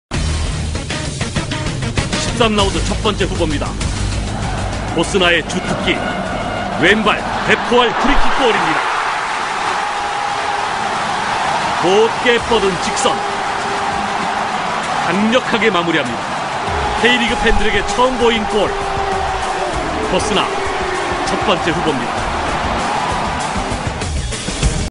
Un Coup Franc a 121 Km/h ENORME
Eddy Bosnar, défenseur australien des Suwon Bluewings Samsung (K-League), a inscrit un coup franc missile chronométré à 121 km/h